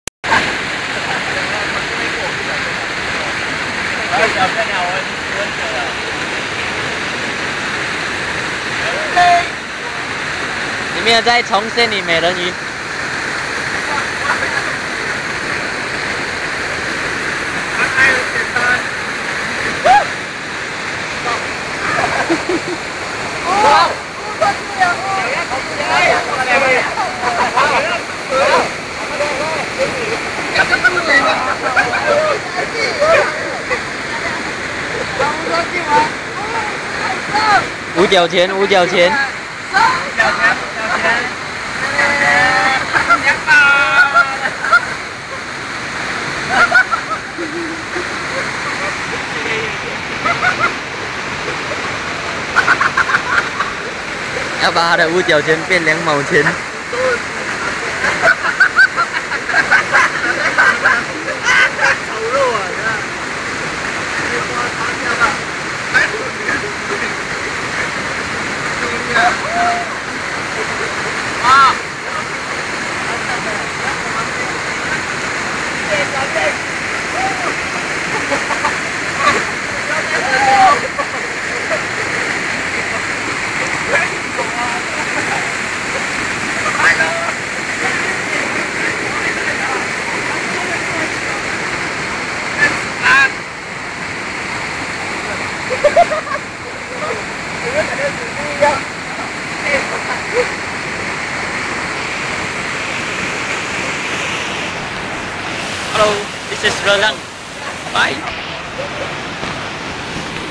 GunungLedang1daytrip.mp4
Was having fun with my friends at Gunung Ledang, Malaysia. And this video is to keep our memory fresh whenever we want to recall back the sweet memories.